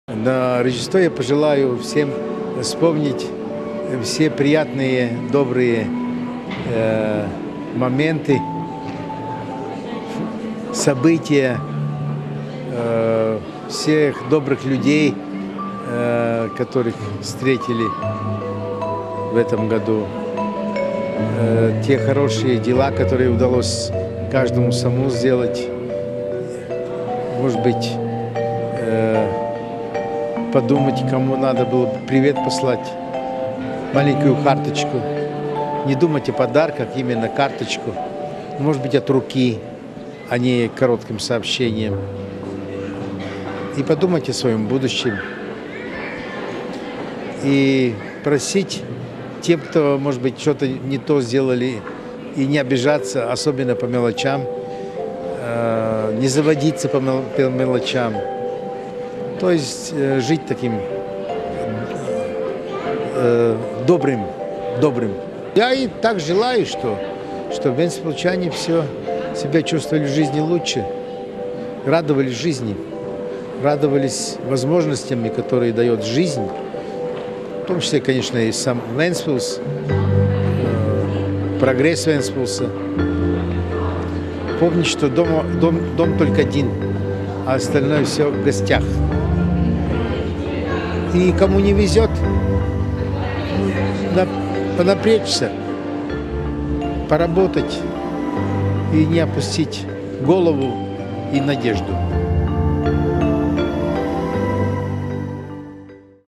Новогоднее поздравление Айварса Лембергса
Председатель Вентспилсской городской думы Айварс Лембергс поздравляет вентспилсчан с новым 2014 годом.